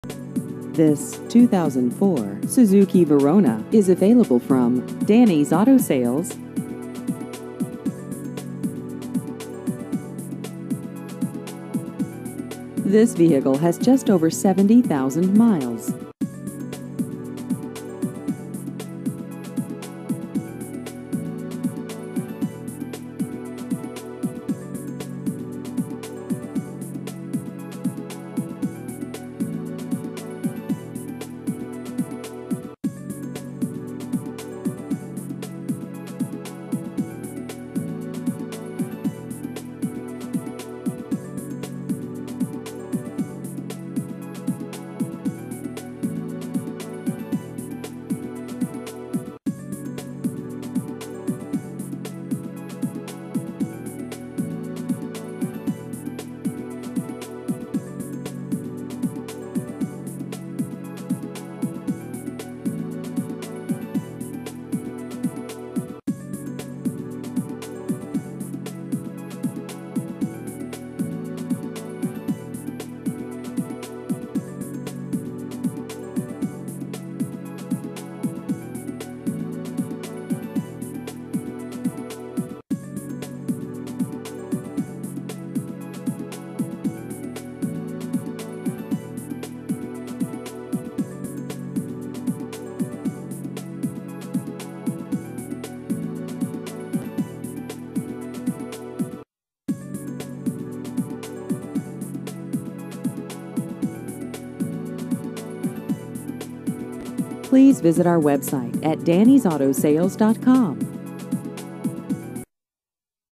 2004 Suzuki Verona Used Cars Philadelphia PA
This 2004 Suzuki Verona is available from Dani's Auto Sales.